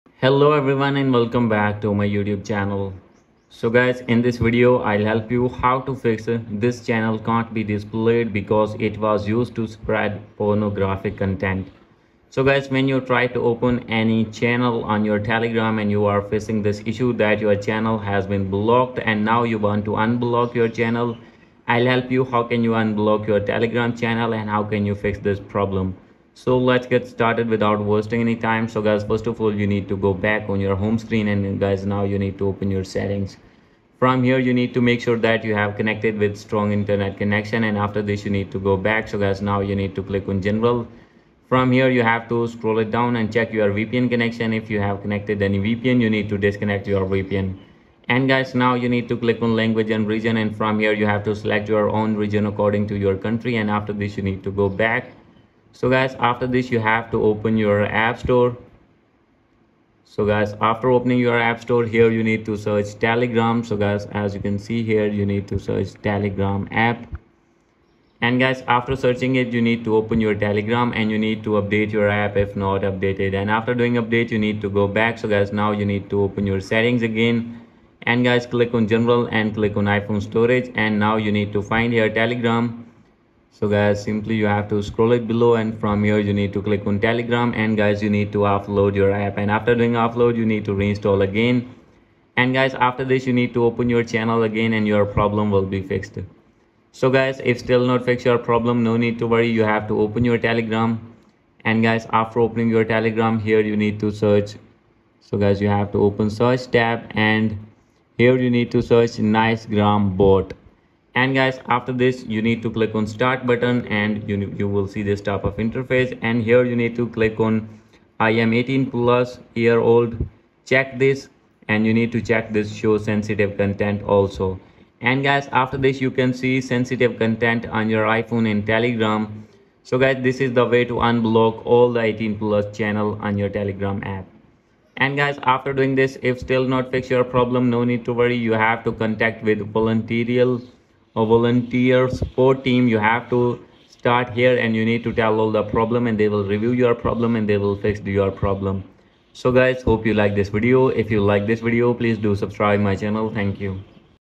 How to Fix "This Channel Can't be Displayed" on Telegram | How to Unblock Telegram Channels
Are you encountering the "This Channel Can't be Displayed" error on Telegram? 📱❌ In this video, we'll guide you through the steps to fix this issue and regain access to your favorite Telegram channels. Follow our detailed tutorial to resolve this problem quickly and easily.